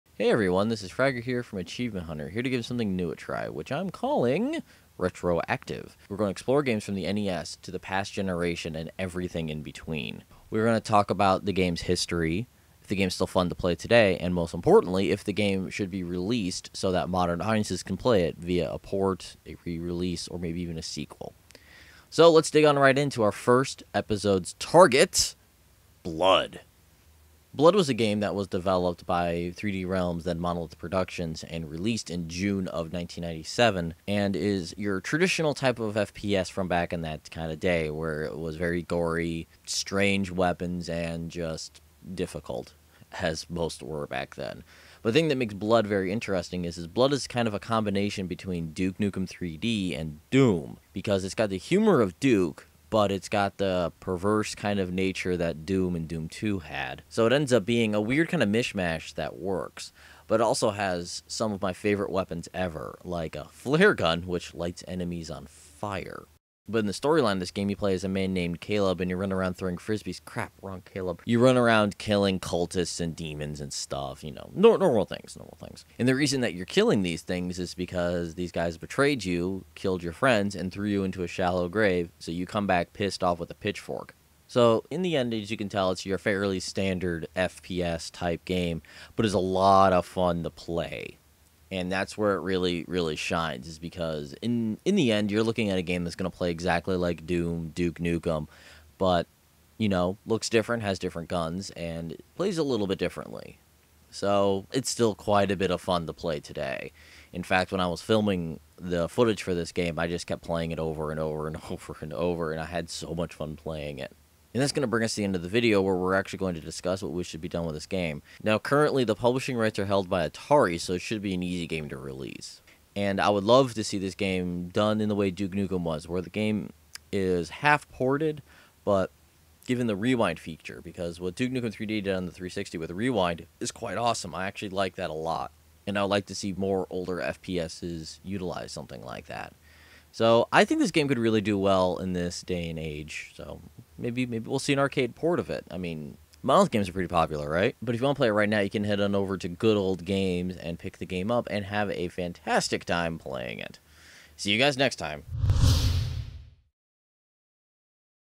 Retro Active - Blood | Rooster Teeth
Fragger takes a look a little further back than the typical ALBA, and remembers Blood, the gory FPS from yesteryear.